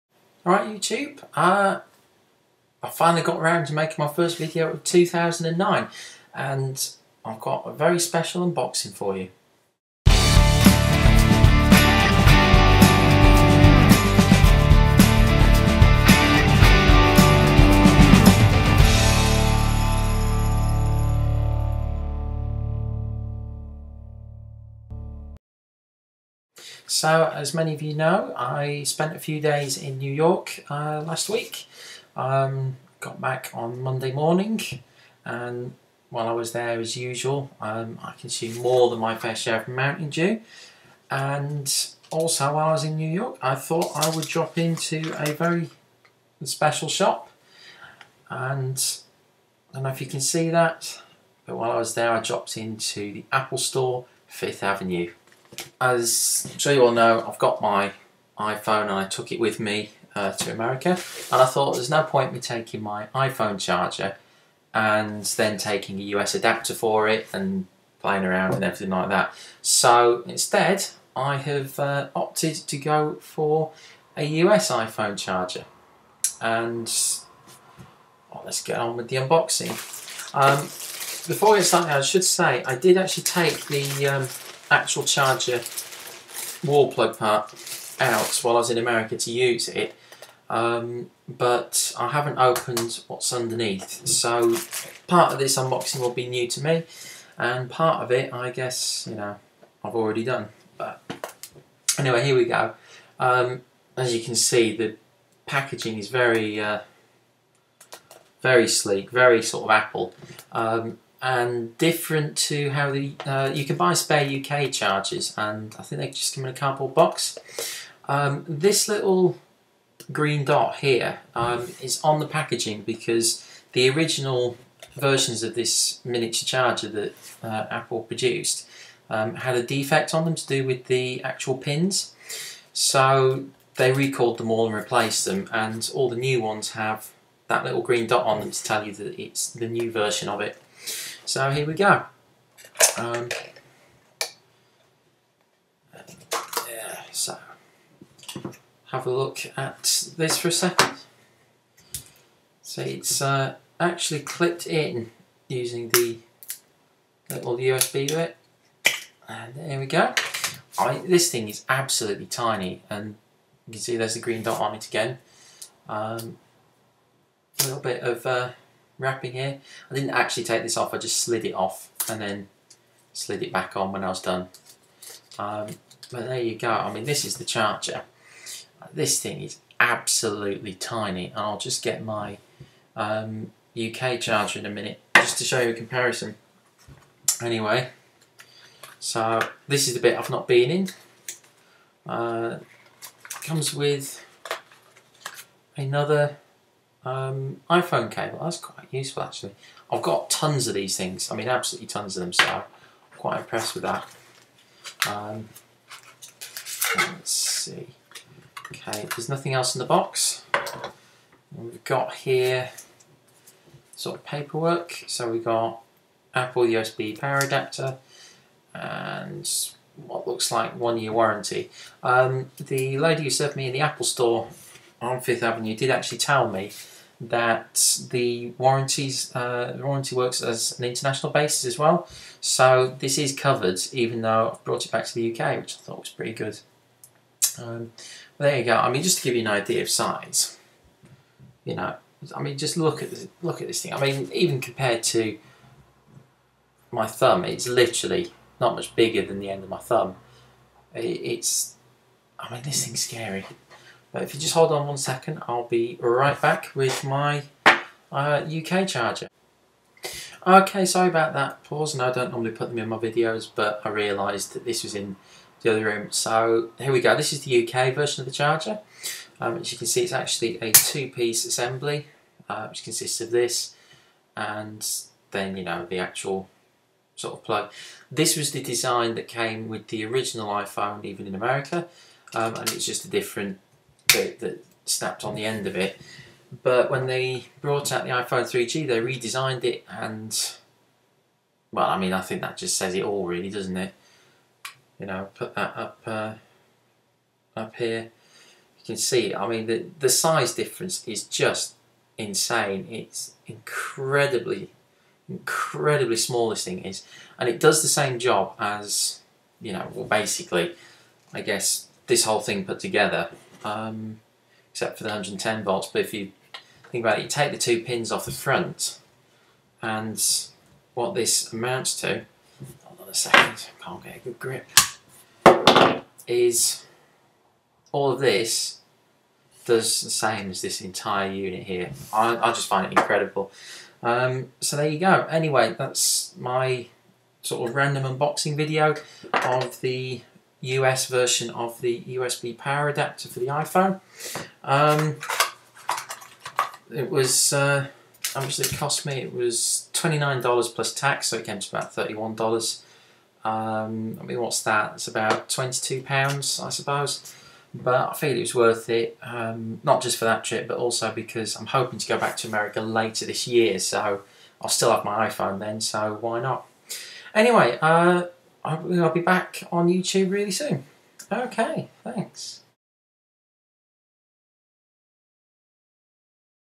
US iPhone charger unboxing
Hey everyone, while I was in New York I picked up a US charger for my iPhone. Here is the unboxing ;-)